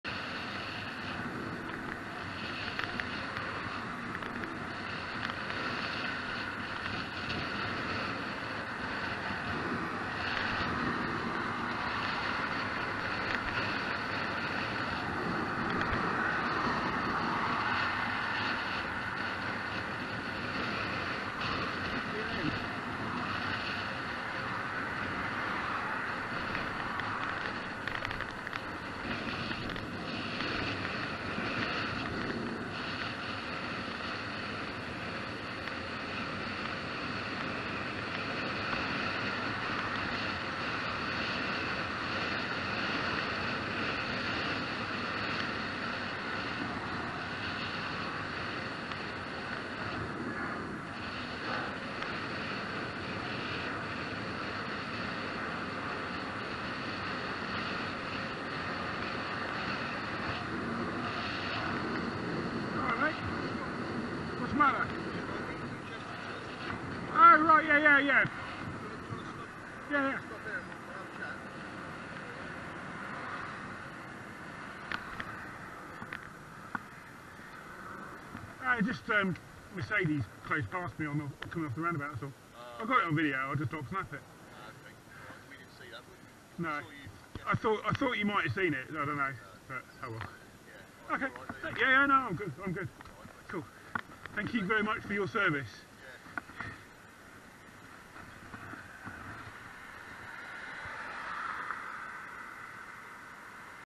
e-bike Pulled over by the police! (C20MJW close pass)
C20MJW close pass

after a close pass i waved at a passing police car thinking they might have seen it, they turned round and caught me up to find out why I gestured at them and to make sure I was ok.

Police do a great job, just need more of them!

PS my e-bike is 100% uk road legal.